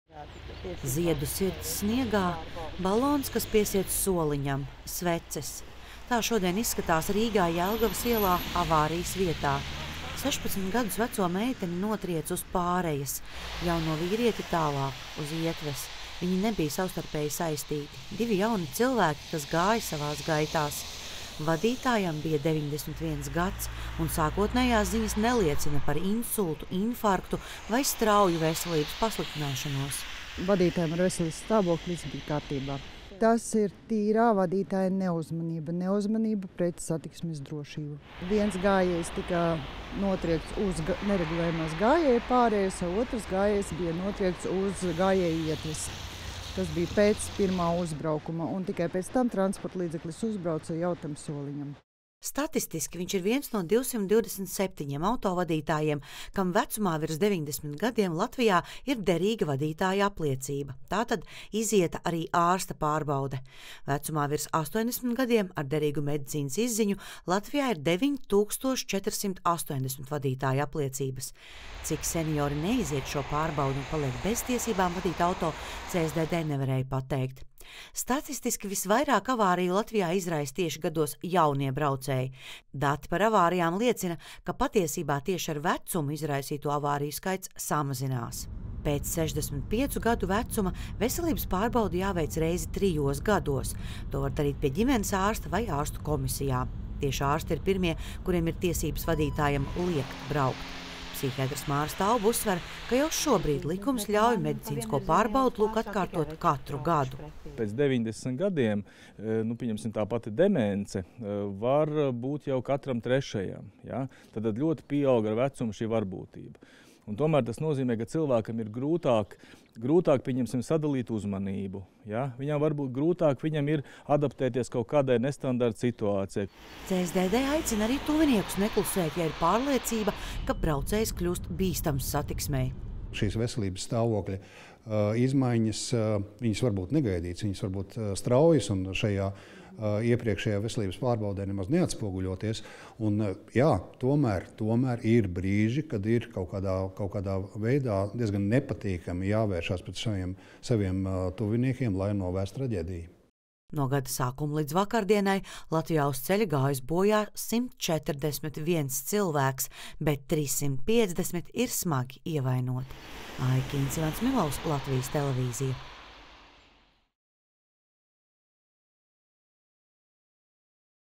Torņakalna traģiskā avārija – klaja neuzmanība
Klaja neuzmanība! Tādi ir izmeklētāju pirmie secinājumi par traģisko avāriju sestdien Torņakalnā. Tur 91 gadu vecs autovadītājs uz gājēju pārejas notrieca 16 gadus vecu meiteni, bet pēc tam uztriecās arī jaunietim uz ietves. Meitene mira, vīrietis ar traumām ārstējas slimnīcā. Notikušais uzjundījis jautājumus par to, vai cilvēkiem lielā vecumā jāsēžas pie stūres un vai ir veiktas visas medicīniskās pārbaudes. Šajā gadījumā gan braukšanas apliecība bija derīga, jo bija izieta medicīniskā pārbaude. Un arī CSDD dati liecina, ka ne seniori, bet tieši gados jauni braucēji avārijas izraisa visbiežāk. Taču veselības problēmas gan cilvēkus gados piemeklē biežāk. Tāpēc kopš 65 gadu vecuma veselības pārbaude jāveic reizi trijos gados. Un, ja ārstam ir aizdomas, ka kaut kas nav kārtībā, atkārtotu pārbaudi var nozīmēt jau pēc gada.